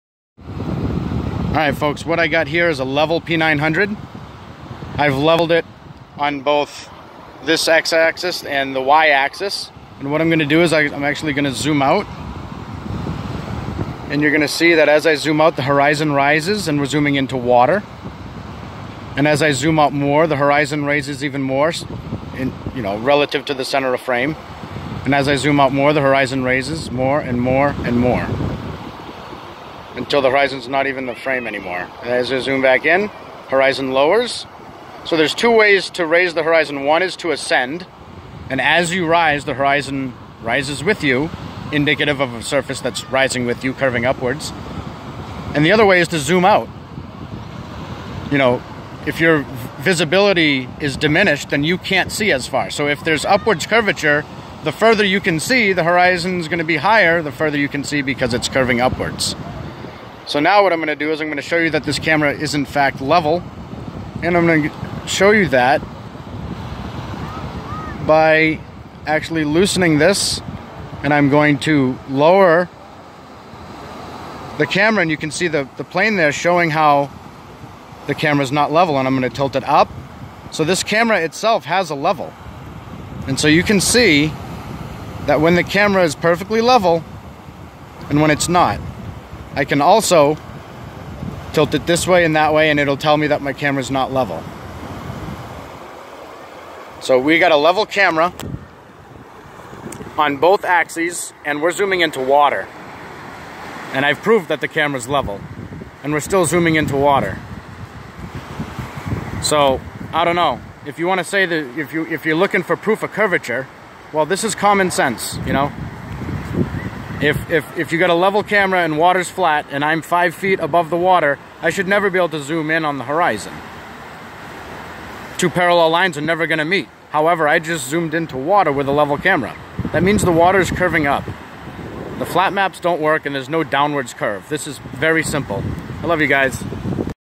Concave Earth Uprising
Zooming raises the horizon even more, with a level P900, on both horizontal axis.

Tybee Island
Savannah, Georgia